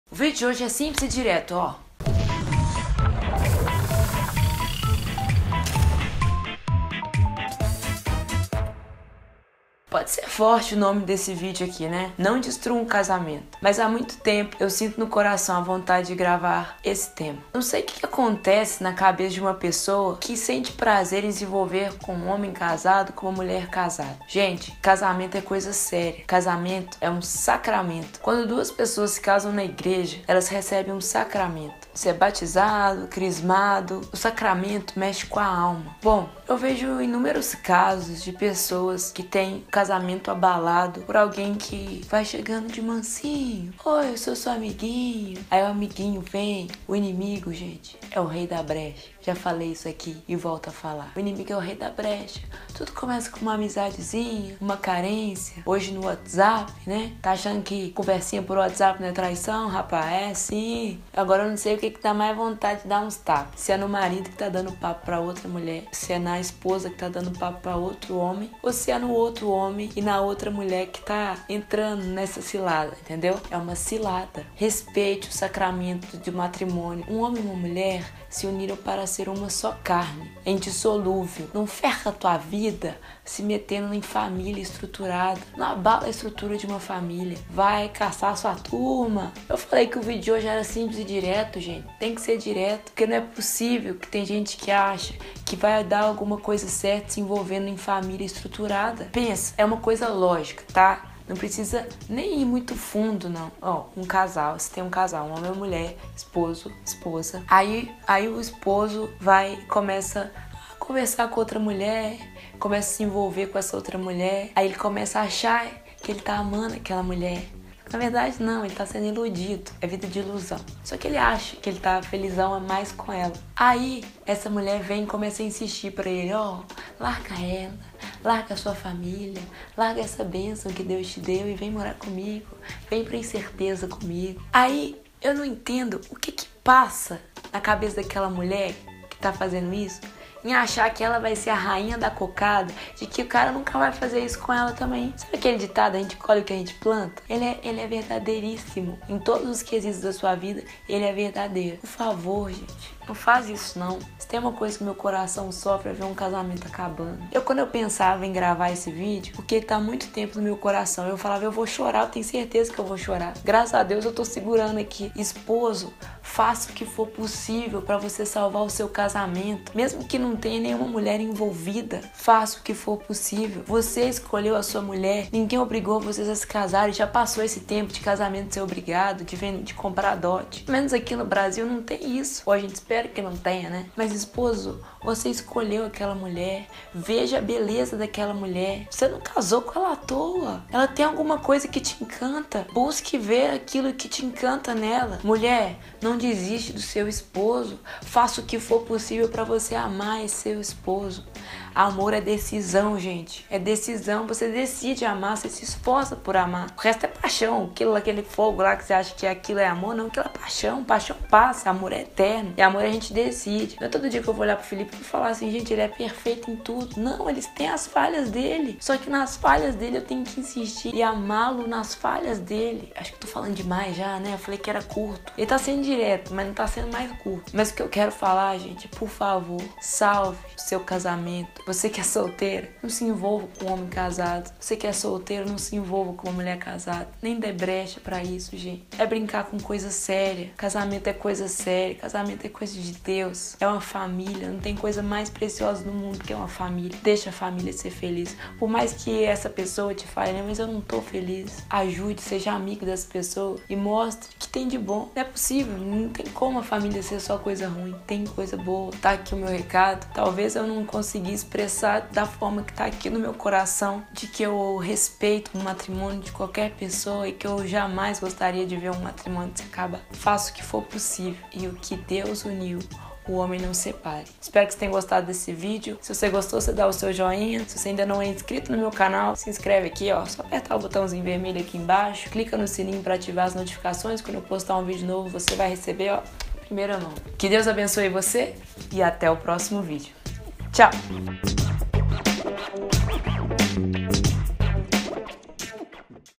Não destrua um casamento
Parece que tem gente que tem prazer de abalar as estruturas de uma família feliz. O inimigo quer destruir as famílias, mas nós precisamos fazer diferente e mostrar o valor de uma família. O que Deus uniu, o homem não separe